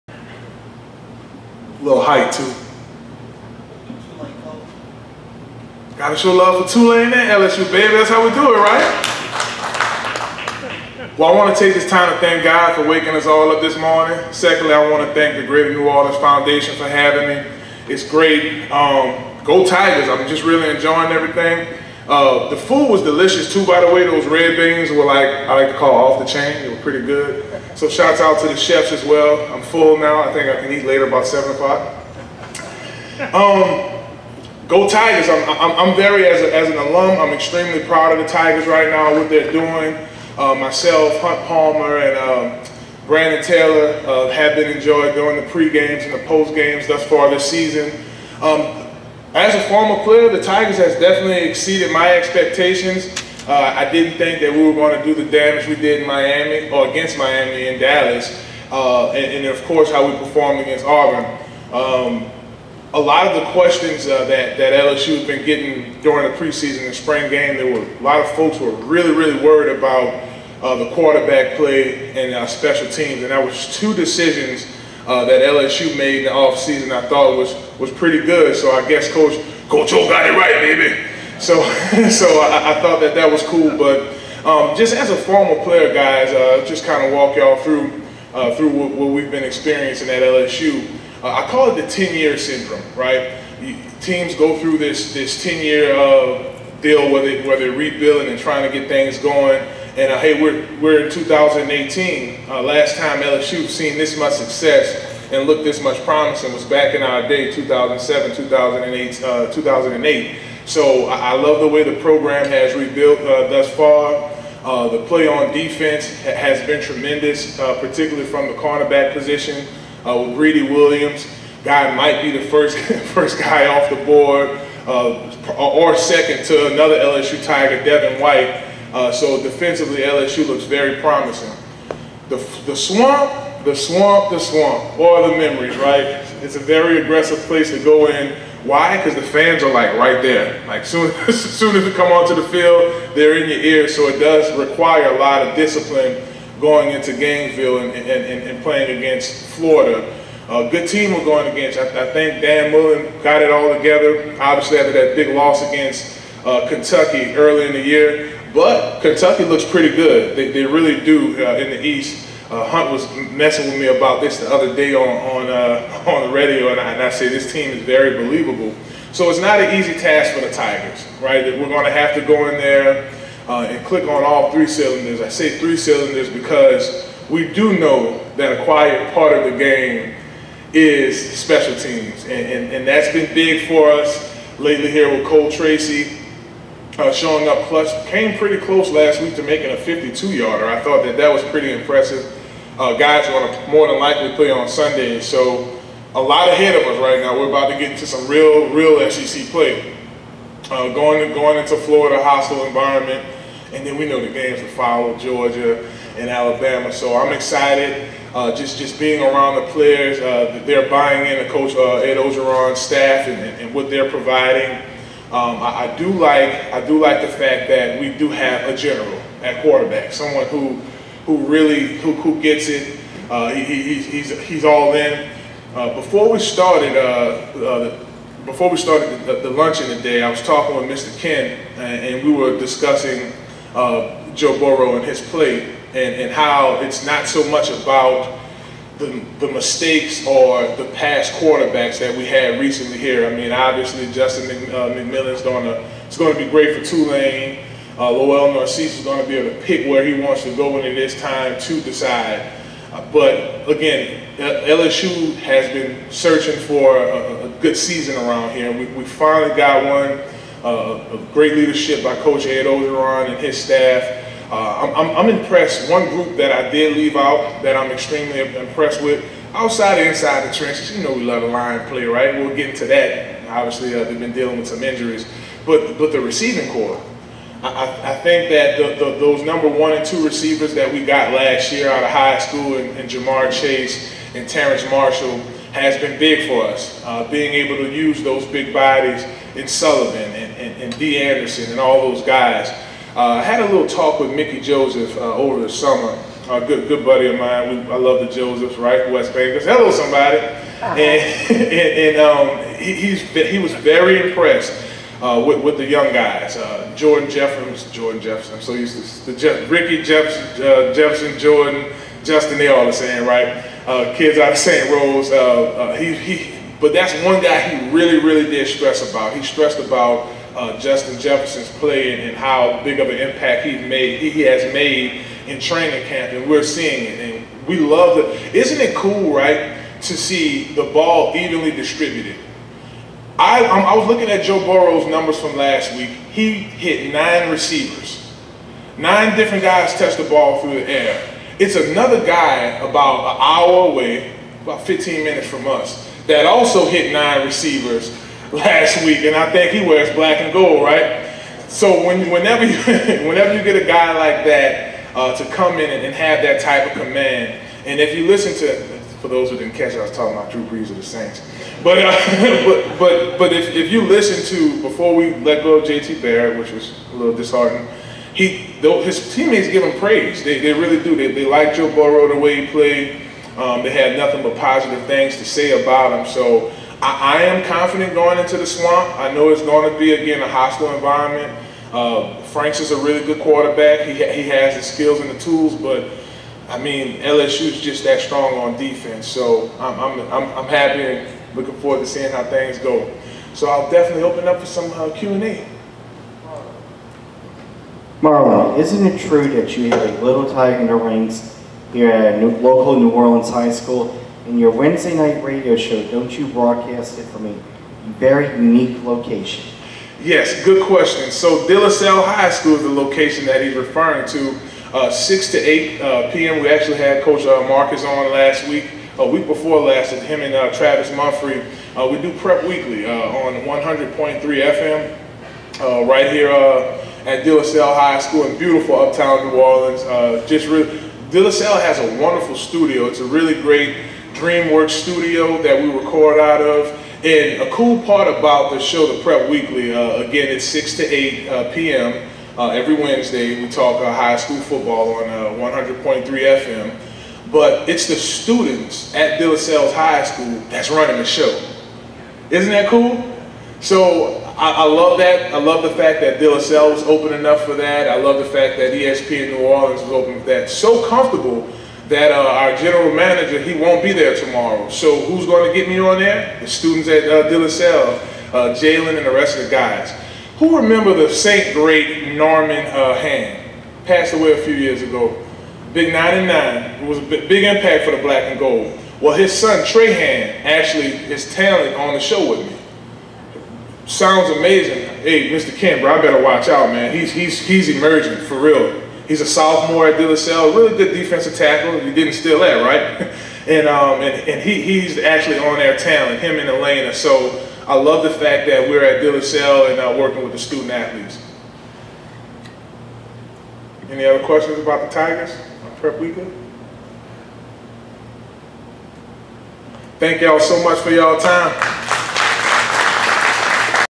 Marlon Favorite talks LSU football
Former Tigers star discusses the 5-0 LSU team at the GNOSF QB Club inside Rock n' Bowl.